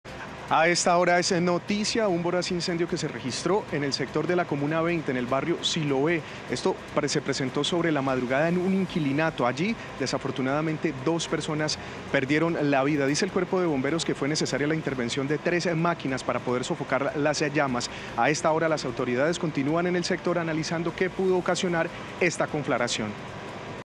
Dos personas mueren durante voraz incendio en inquilinato de Cali
Un incendio que tuvo lugar la madrugada de este 6 de julio en un inquilinato del sector de Siloé, en Cali, cobró la vida de dos personas. De acuerdo con primeras versiones, las víctimas son un hombre y una mujer.